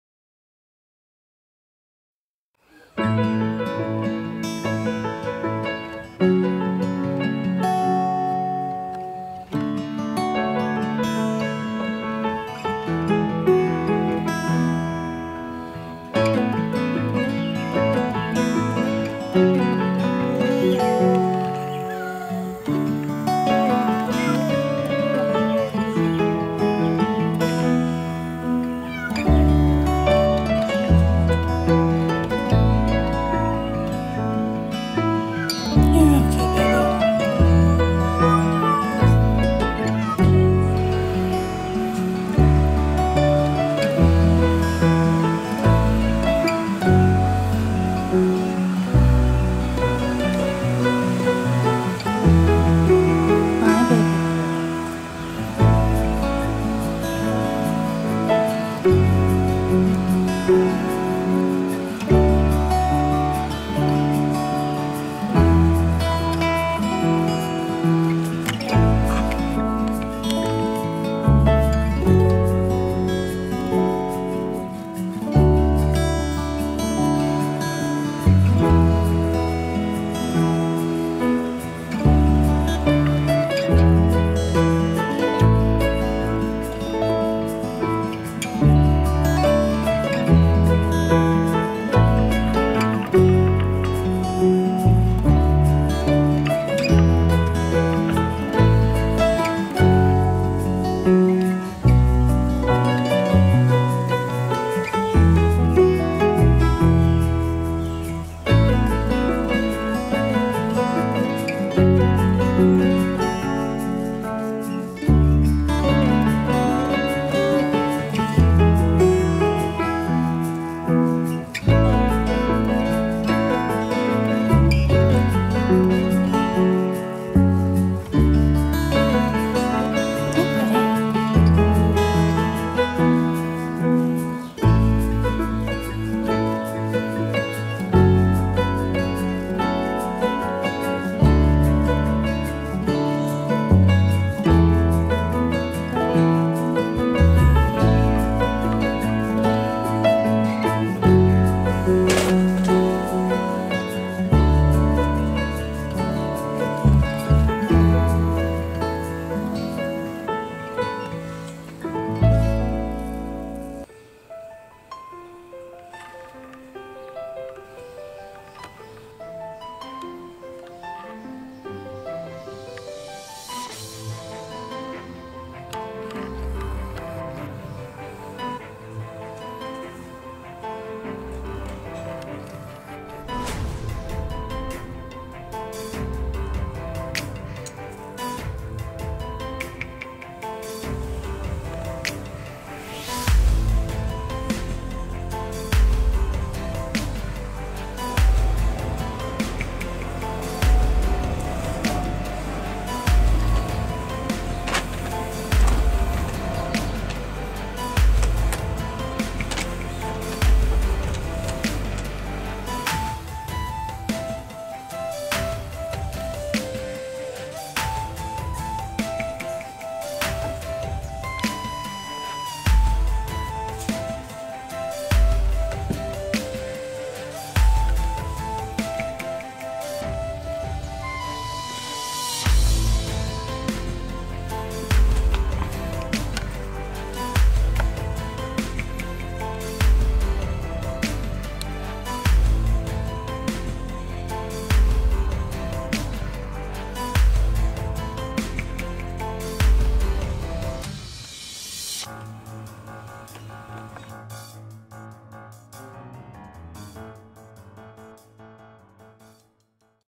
8 Ragdoll Kitties Visit The Gazebo!
I think it went fairly well for their first visit into the great unknown. Fresh air and ohhhh so much wildlife!